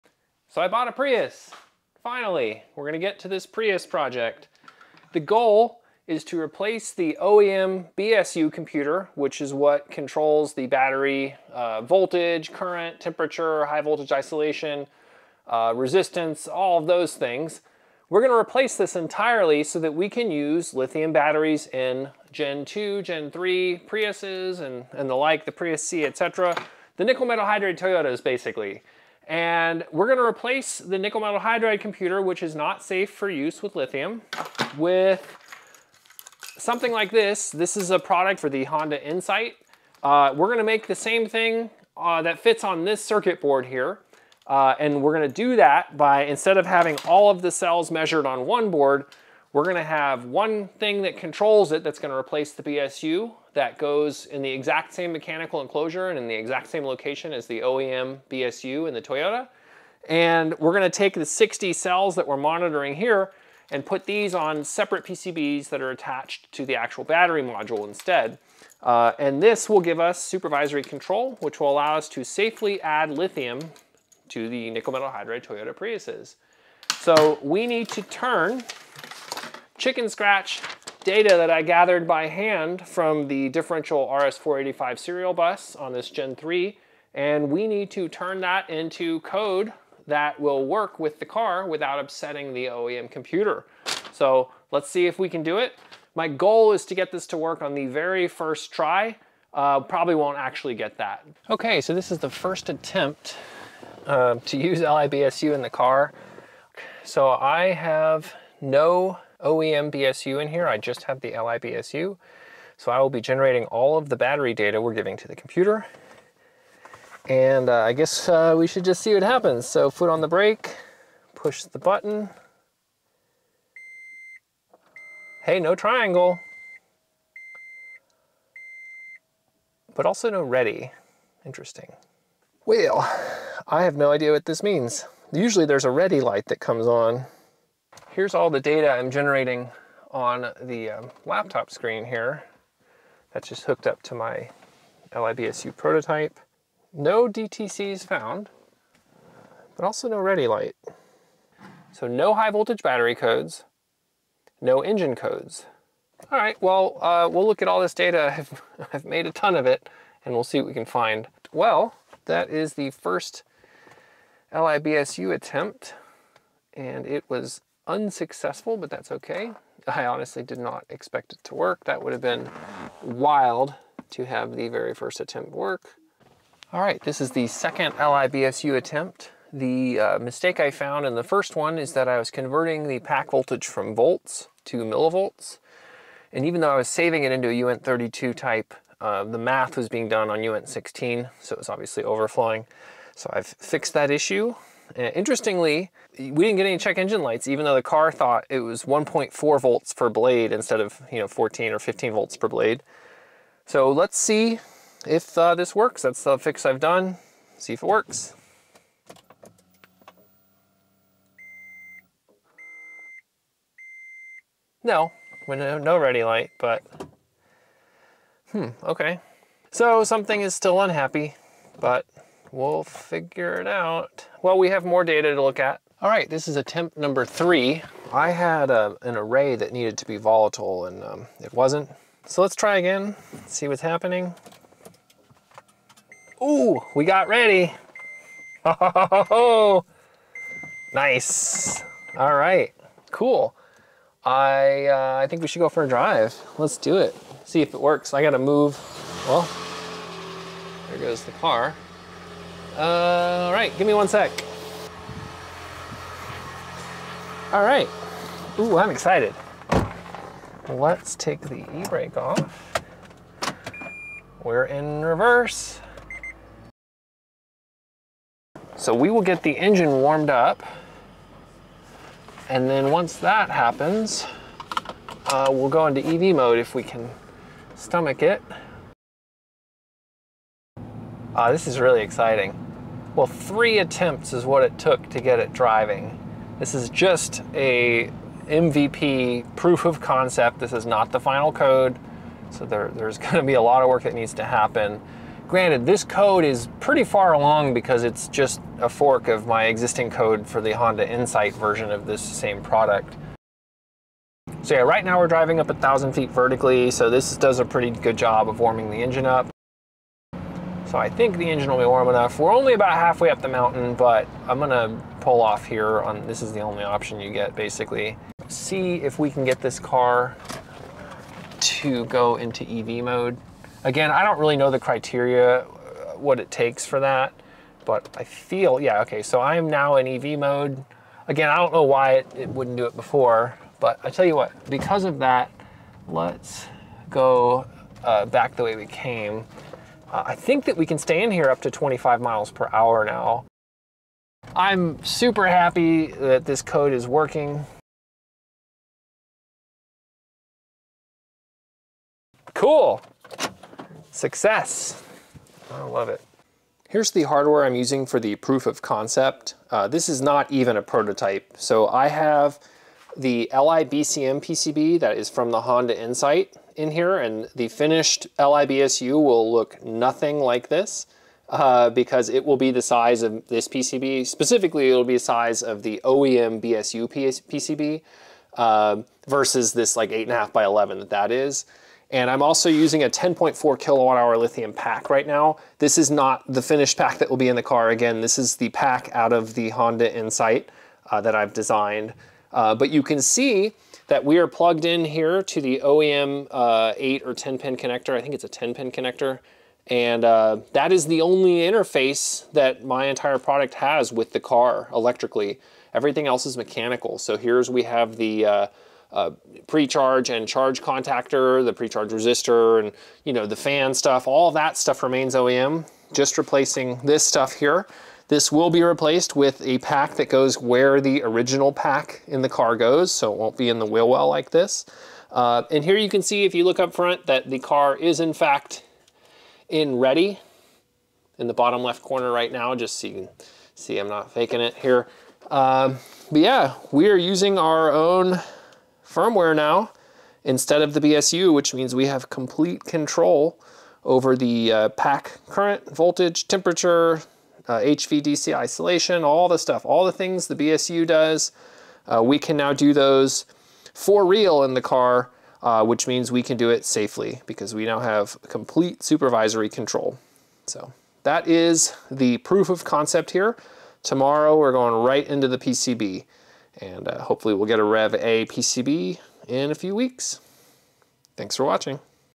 LiBSU Proof of Concept - Gen3 Toyota Prius Lithium BMS Computer
Working hardware/firmware proof of concept that replaces the NiMH BSU computer with a safe Lithium BMS with full supervisory control.